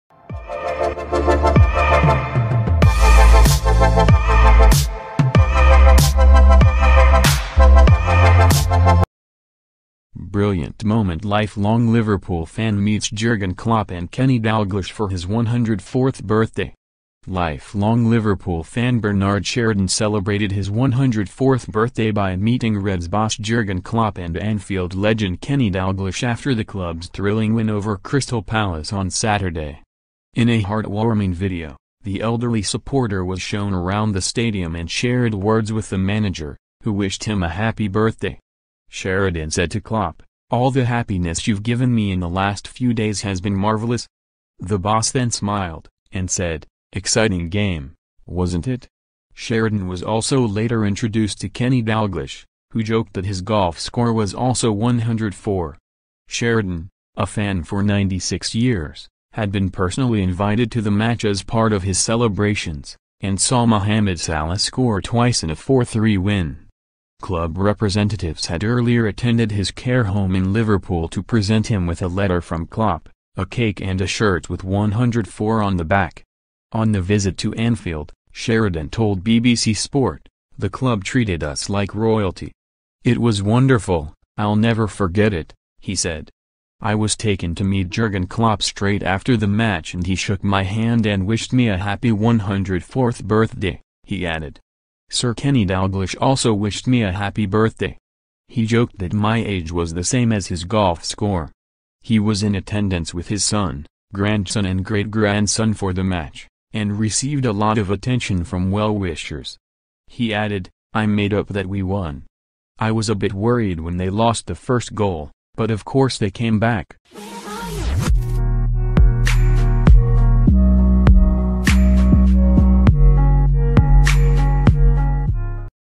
Brilliant moment lifelong Liverpool fan meets Jurgen Klopp and Kenny Dalglish for his 104th
Brilliant moment lifelong Liverpool fan meets Jurgen Klopp and Kenny Dalglish for his 104th birthday. Lifelong Liverpool fan Bernard Sheridan celebrated his 104th birthday by meeting Reds boss Jurgen Klopp and Anfield legend Kenny Dalglish after the club's thrilling win over Crystal Palace on Saturday.

In a heartwarming video, the elderley supporter was shown around the stadium and shared words with the manager, who wished him a happy birthday.

Sheridan said to Klopp: 'All the happiness you've given me in the last few days has been marvellous.'

The boss then smiled, and said: 'Exciting game, wasn't it?'

Sheridan was also later introduced to Kenny Dalglish, who joked that his golf score was also 104.

Sheridan, a fan for 96 years, had been personally invited to the match as part of his celebrations, and saw Mohamed Salah score twice in a 4-3 win.

Club representatives had earlier attended his care home in Liverpool to present him with a letter from Klopp, a cake and a shirt with '104' on the back.

On the visit to Anfield, Sheridan told BBC Sport: 'The club treated us like royalty... it was wonderful, I'll never forget it,' he said.

'I was taken to meet Jurgen Klopp straight after the match and he shook my hand and wished me a happy 104th birthday,' he added.

'Sir Kenny Dalglish also wished me a happy birthday. He joked that my age was the same as his golf score.'

He was in attendance with his son, grandson and great-grandson for the match, and received a lot of attention from well-wishers.

He added: 'I'm made up that we won. I was a bit worried when they lost the first goal, but of course they came back.'